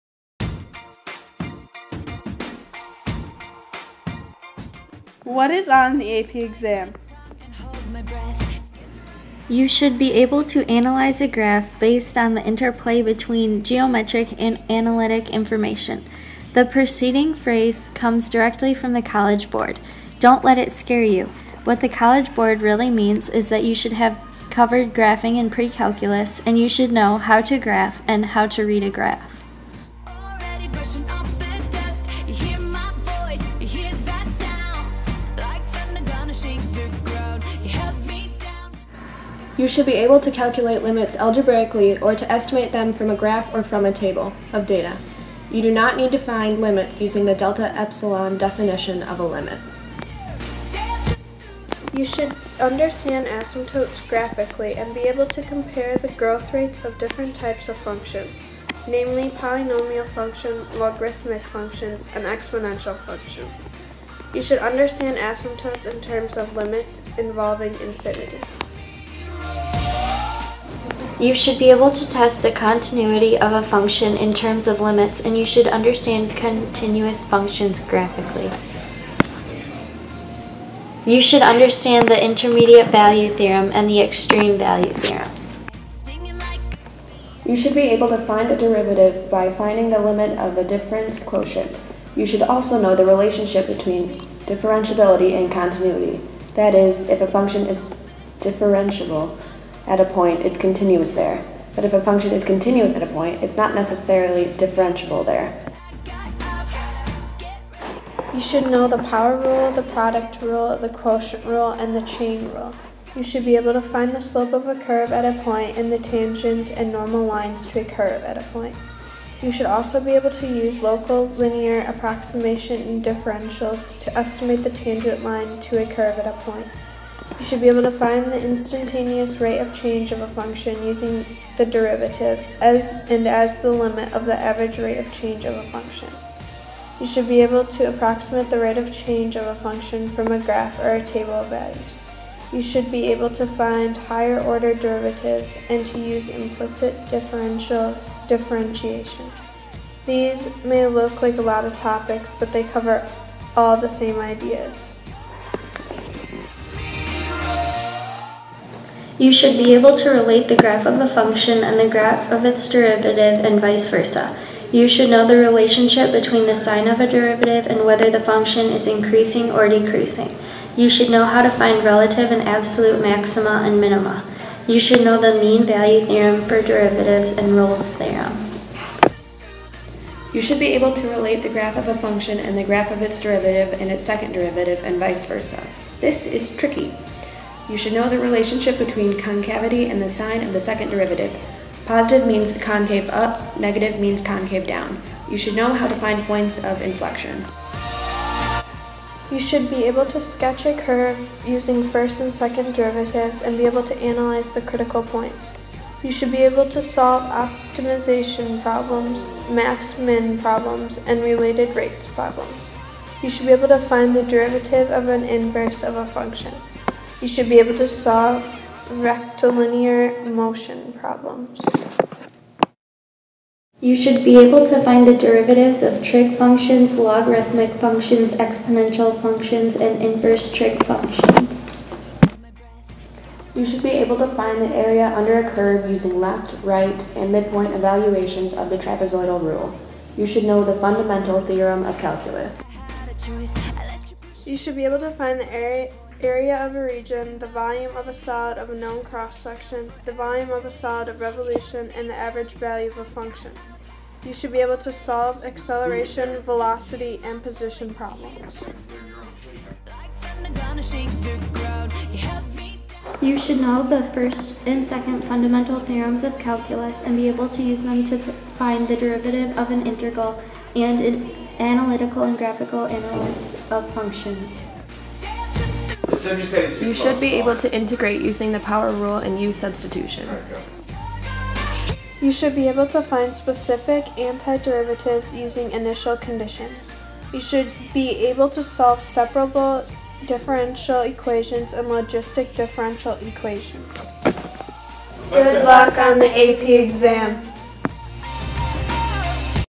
What's on the AP Calculus Exam
Some of LMC's finest explain what topics will be on the AP Calculus Exam